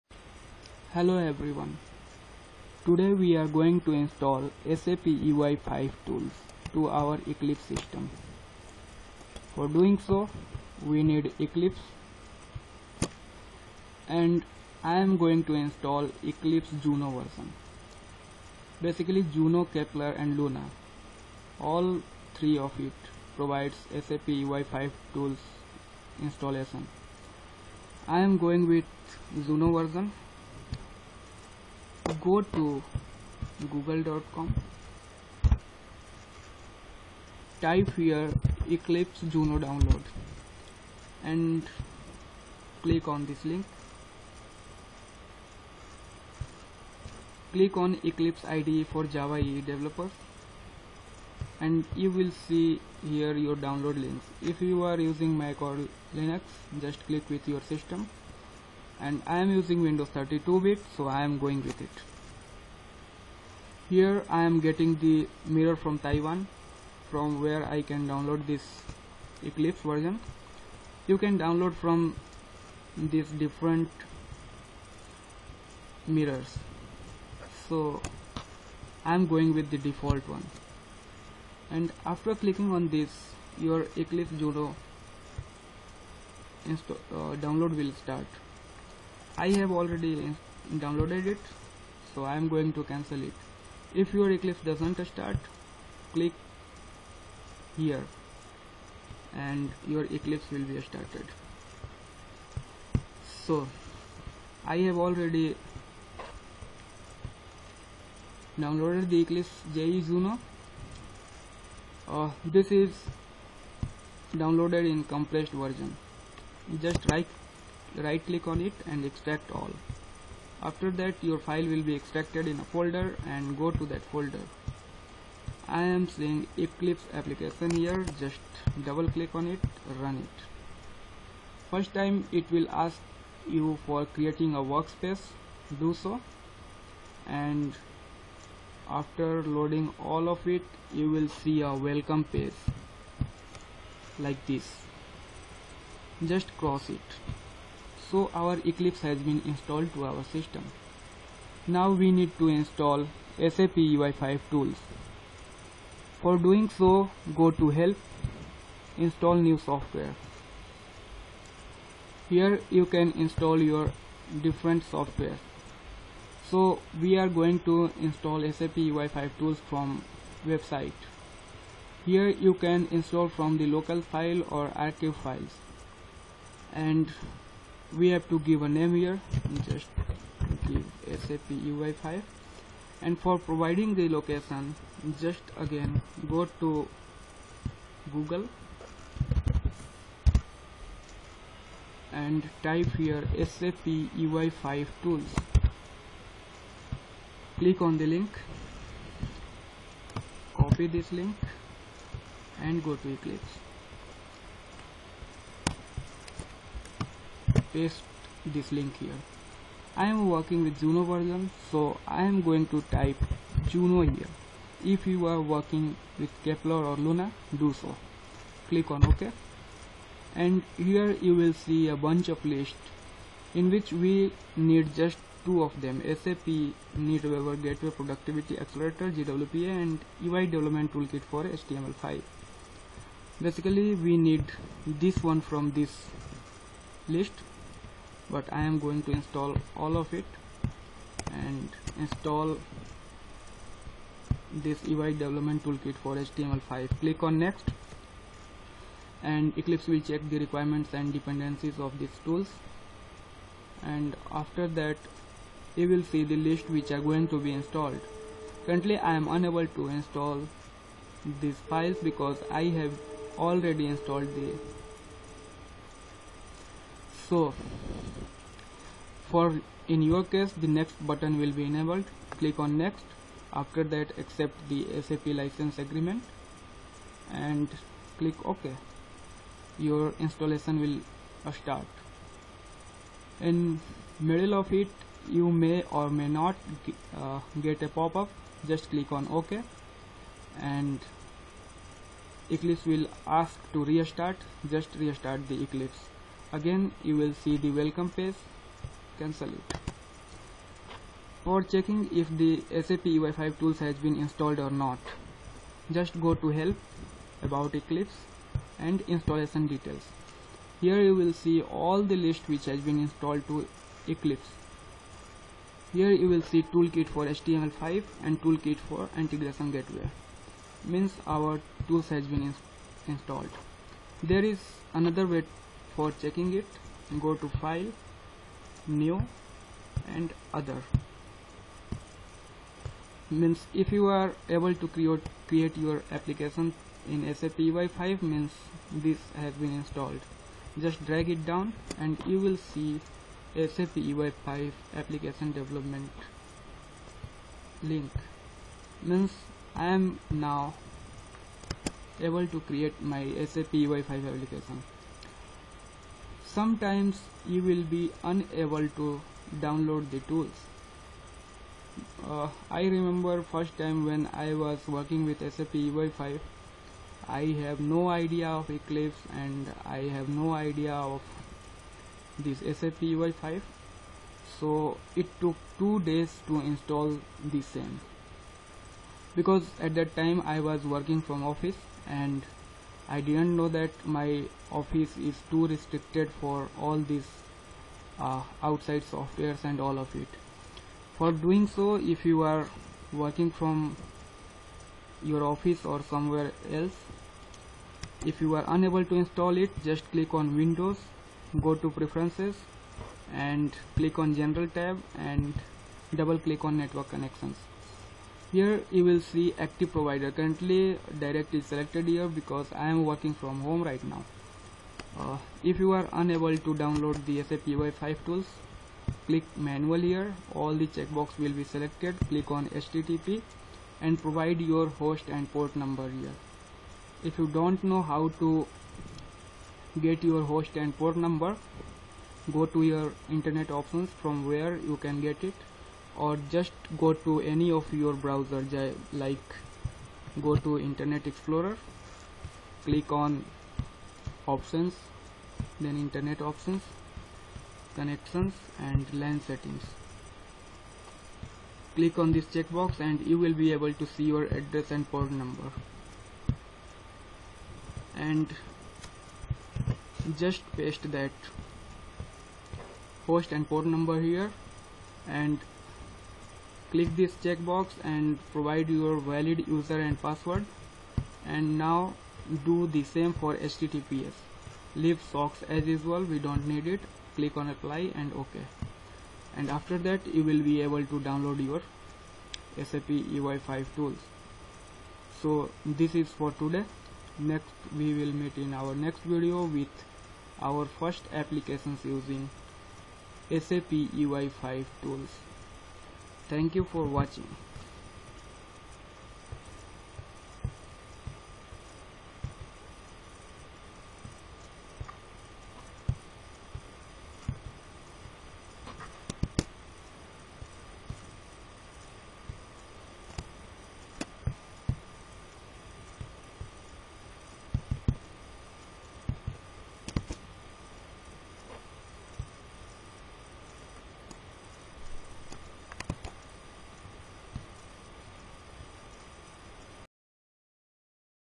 SAPUI5 - Installation
This video is about how to install SAPUI5 tools to eclipse.